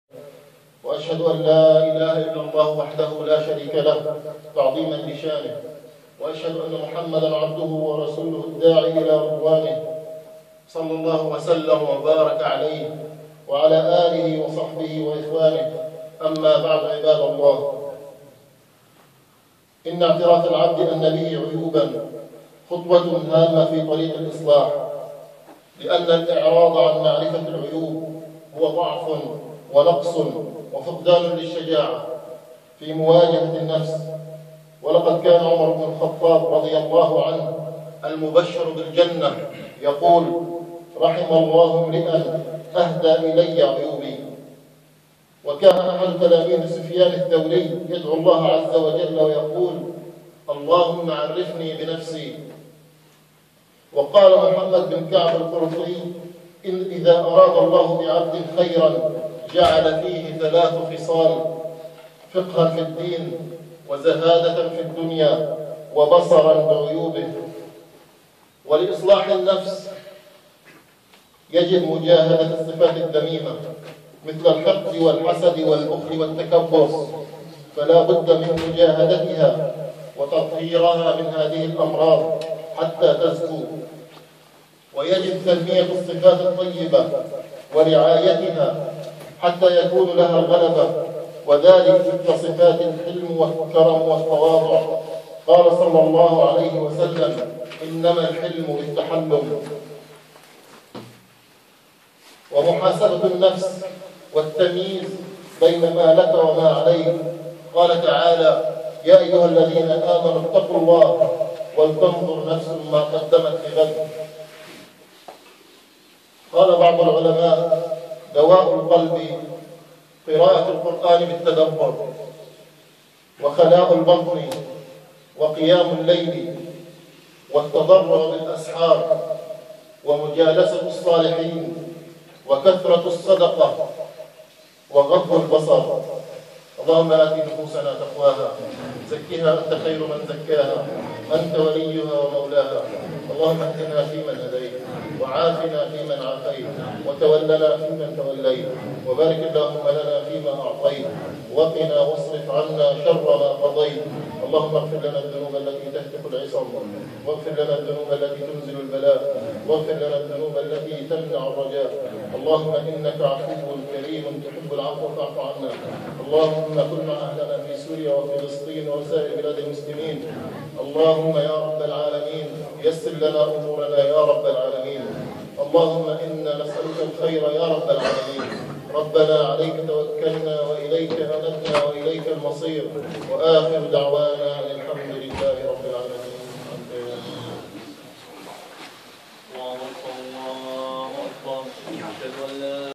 صلاة وخطبة الجمعة في تريبودا
خطيب الجمعة أبو باسل